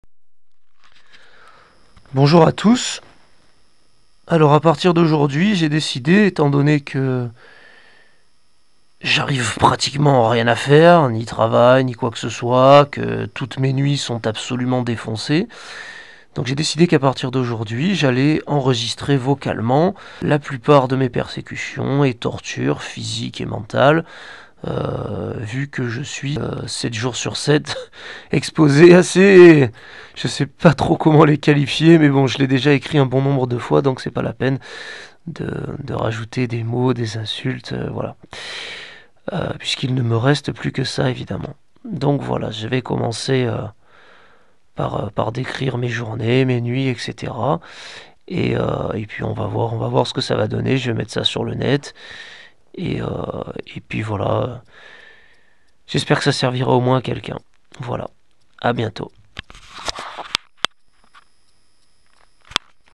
BruléParlesilluminés - Audio 1 - Décision de faire des enregistrements audio - 190512
Il est autour de 3 heures du mat quand je me décide, alors que mes nuits continuent d'être détruites, que l'on me ferme le monde, que je n'ai pas d'emploi, pas de petite amie et plus grand-chose d'autre, à réaliser des enregistrements audio d'explications de mon énorme et incroyable problème. Mes descriptions seront semblables à celles que j'ai déjà réalisées par écrit sur mon blog, pour ceux qui les auraient déjà parcourues. C'est un exercice qui n'est pas simple du tout d'autant plus au vu de l'état dans lequel je me trouve aujourd'hui. Je fais quand-même l'effort en espérant que cela servira positivement quelqu'un. Le net venait tout juste de m'être coupé pour les deux semaines à venir j'en ai profité pour faire des enregistrements.

(La vidéo dure normalement 1:02 mais là elle dure 1:03, c'est donc bien du 13...)